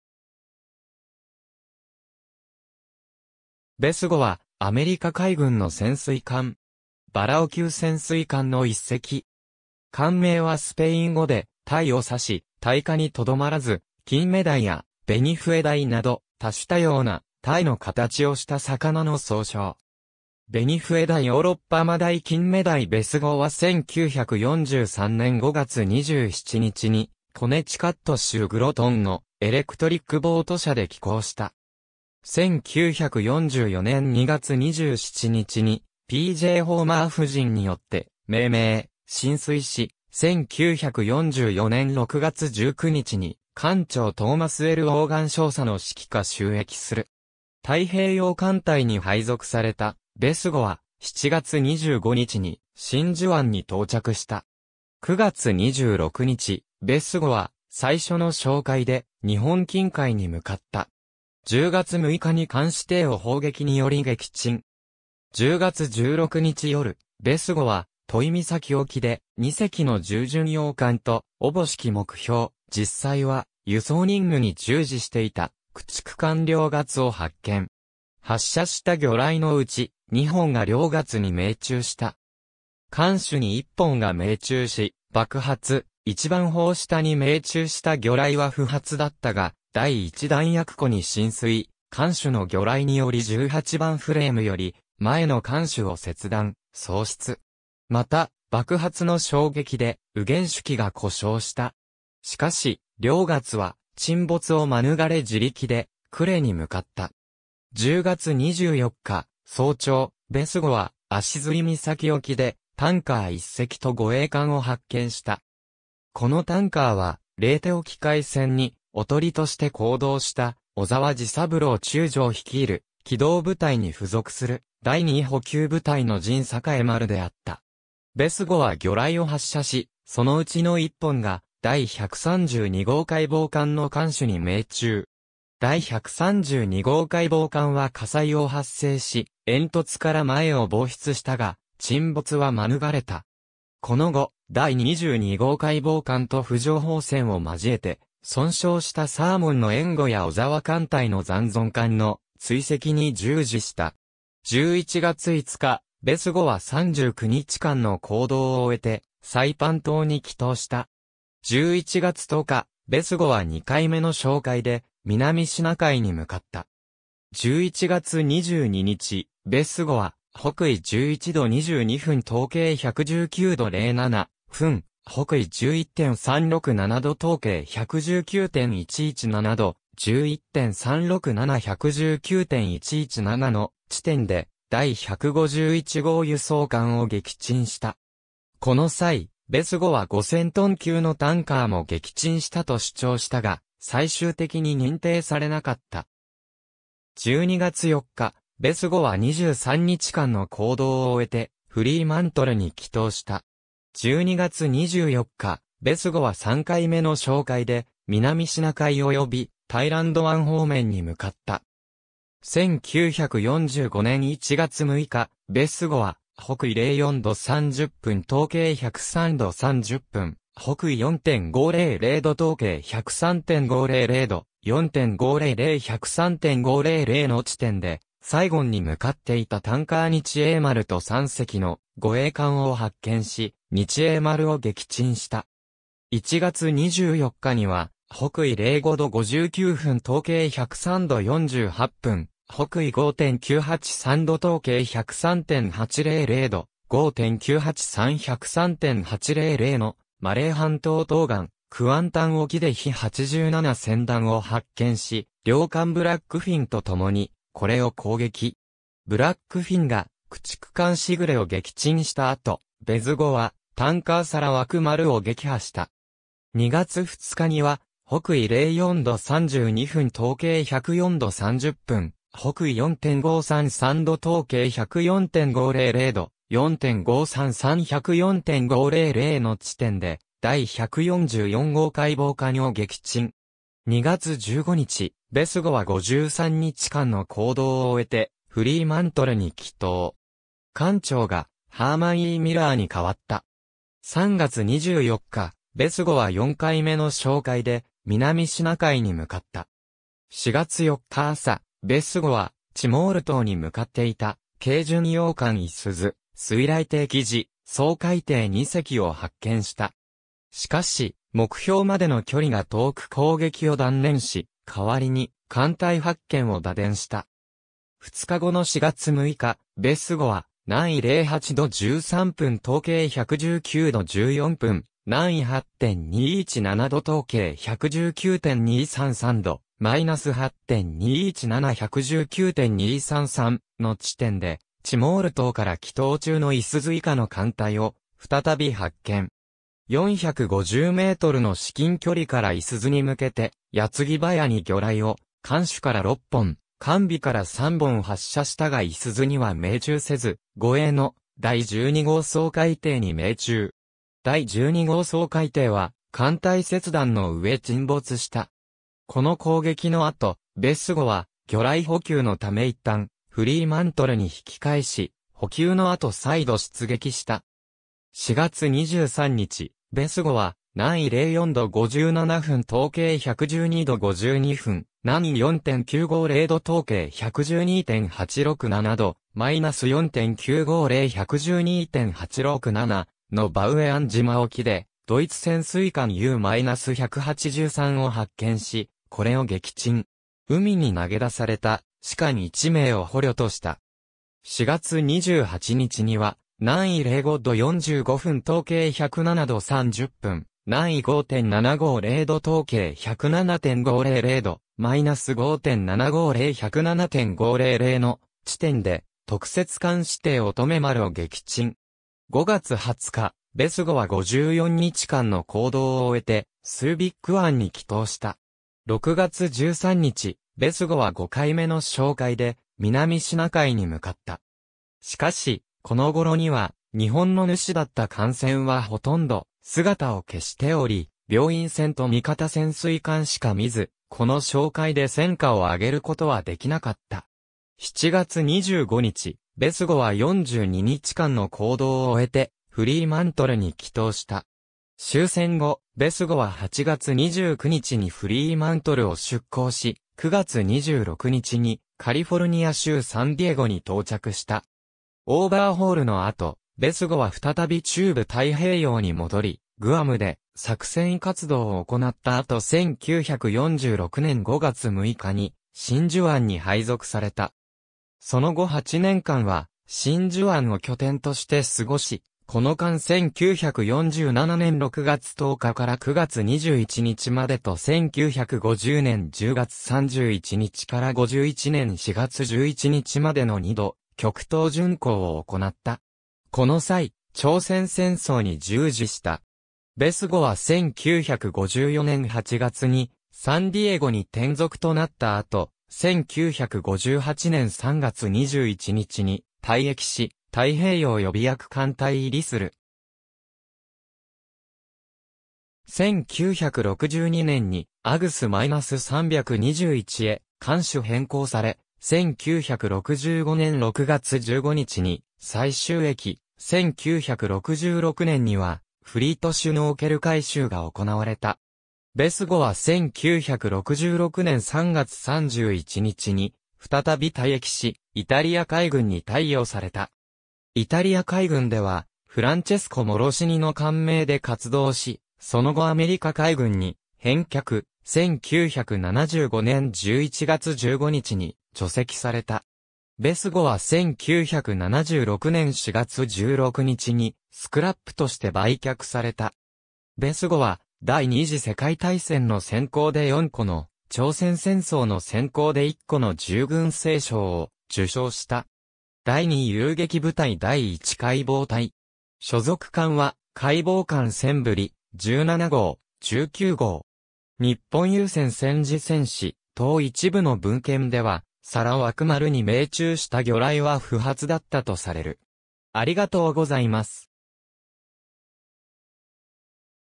ベスゴ (潜水艦)
ベスゴ (USS Besugo, SS/AGSS-321) は、アメリカ海軍の潜水艦。
艦名はスペイン語で「タイ」を指し、タイ科にとどまらずキンメダイやベニフエダイなど多種多様な「タイの形をした魚」の総称。
ベニフエダイ（プエルトリコの通称Besugo） ヨーロッパマダイ（ベネズエラの通称Besugo） キンメダイ（スペインの通称Besugo） ベスゴは1943年5月27日にコネチカット州グロトンのエレクトリック・ボート社で起工した。
1944年2月27日にP・J・ホーマー夫人によって命名、進水し、1944年6月19日に艦長トーマス・L・ウォーガン少佐（アナポリス1930年組）の指揮下就役する。
太平洋艦隊に配属されたベスゴは、7月25日に真珠湾に到着した。
9月26日、ベスゴは最初の哨戒で日本近海に向かった。
10月6日に監視艇を砲撃により撃沈。
10月16日夜、ベスゴは都井岬沖で、2隻の重巡洋艦と思しき目標、実際は輸送任務に従事していた駆逐艦涼月を発見。
発射した魚雷のうち、2本が涼月に命中した。
艦首に1本が命中し爆発、 1番砲下に命中した魚雷は不発だったが第1弾薬庫に浸水、艦首の魚雷により18番フレームより前の艦首を切断、喪失。
また、爆発の衝撃で右舷主機が故障した。
しかし、涼月は沈没を免れ自力で呉に向かった。
10月24日早朝、ベスゴは足摺岬沖でタンカー1隻と護衛艦を発見した。
このタンカーはレイテ沖海戦に囮として行動した小沢治三郎中将率いる機動部隊に付属する第二補給部隊の仁栄丸（日東汽船、10,241トン）であった。
ベスゴは魚雷を発射し、そのうちの1本が第132号海防艦の艦首に命中。
第132号海防艦は火災を発生し、煙突から前を亡失したが沈没は免れた。
この後、第22号海防艦と浮上砲戦を交えて損傷したサーモン (USS Salmon, SS-182) の援護や小沢艦隊の残存艦の追跡に従事した。
11月5日、ベスゴは39日間の行動を終えてサイパン島に帰投した。
11月10日、ベスゴは2回目の哨戒で南シナ海に向かった。
11月22日、ベスゴは 北緯11度22分 東経119度07分 / 北緯11.367度 東経119.117度 / 11.367; 119.117の地点で第151号輸送艦を撃沈した。
この際、ベスゴは5,000トン級のタンカーも撃沈したと主張したが、最終的に認定されなかった。
12月4日、ベスゴは23日間の行動を終えてフリーマントルに帰投した。
12月24日、ベスゴは3回目の哨戒で南シナ海およびタイランド湾方面に向かった。
1945年1月6日、ベスゴは 北緯04度30分 東経103度30分 / 北緯4.500度 東経103.500度 / 4.500; 103.500の地点でサイゴンに向かっていたタンカー日栄丸（日東汽船、10,020トン）と3隻の護衛艦を発見し、日栄丸を撃沈した。
1月24日には、 北緯05度59分 東経103度48分 / 北緯5.983度 東経103.800度 / 5.983; 103.800のマレー半島東岸クアンタン沖でヒ87船団を発見し、僚艦ブラックフィン (USS Blackfin, SS-322) とともにこれを攻撃。
ブラックフィンが駆逐艦時雨を撃沈した後、ベズゴはタンカーさらわく丸（三菱汽船、5,135トン）を撃破した。
2月2日には、 北緯04度32分 東経104度30分 / 北緯4.533度 東経104.500度 / 4.533; 104.500の地点で第144号海防艦を撃沈。
2月15日、ベスゴは53日間の行動を終えてフリーマントルに帰投。
艦長がハーマン・E・ミラー（アナポリス1938年組）に代わった。
3月24日、ベスゴは4回目の哨戒で南シナ海に向かった。
4月4日朝、ベスゴはチモール島に向かっていた軽巡洋艦五十鈴、水雷艇雉、掃海艇2隻を発見した。
しかし、目標までの距離が遠く攻撃を断念し、代わりに艦隊発見を打電した。
2日後の4月6日、ベスゴは 南緯08度13分 東経119度14分 / 南緯8.217度 東経119.233度 / -8.217; 119.233の地点で、チモール島から帰投中の五十鈴以下の艦隊を再び発見。
450メートルの至近距離から五十鈴に向けて矢継ぎ早に魚雷を艦首から6本、艦尾から3本発射したが五十鈴には命中せず、護衛の第12号掃海艇に命中。
第12号掃海艇は艦体切断の...